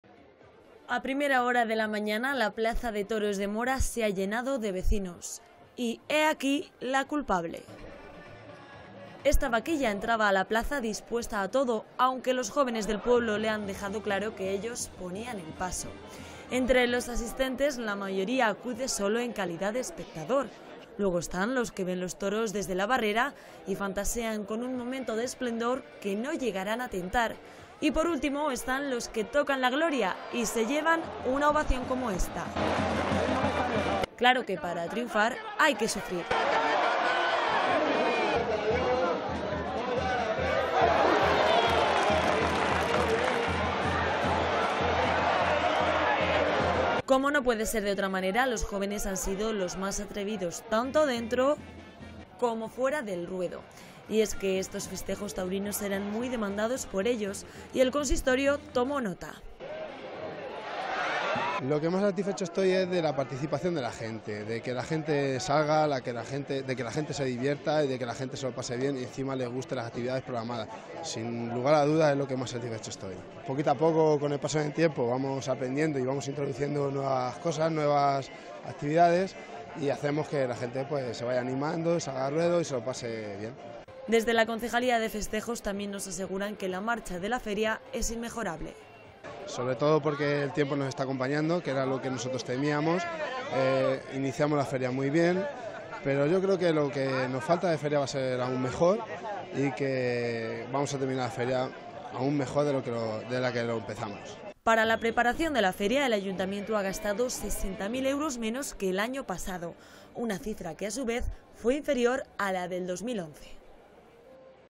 Las vaquillas protagonistas de las Fiestas de Mora
En Mora continúan celebrando su Feria Grande. Hoy martes tenía lugar el último día de vaquillas, una actividad que lleva tres años implantada pero que ya se ha convertido en esencial.
A primera hora de la mañana la Plaza de Toros de Mora se ha llenado de vecinos. Y he aquí la culpable. Esta vaquilla entraba a la plaza dispuesta a todo, aunque los jóvenes del pueblo le han dejado claro que ellos ponían el paso.
Entre los asistentes, la mayoría acude solo en calidad de espectador. Luego están los que ven los toros desde la barrera y fantasean con un momento de esplendor que no llegarán a tentar y, por último, están los que tocan la gloria y se llevan una ovación como esta.
Claro que, para triunfar, hay que sufrir.
Como no puede ser de otra manera, los jóvenes han sido los más atrevidos dentro. Y fuera del ruedo. Y es que estos festejos taurinos eran muy demandados por ellos, y el consistorio tomó nota.
Desde la concejalía de festejos también nos aseguran que la marcha de la feria es inmejorable.
Para la preparación de estos festejos el ayuntamiento ha gastado 60 mil euros menos que el año pasado, una cifra que, a su vez, fue inferior a la del 2011.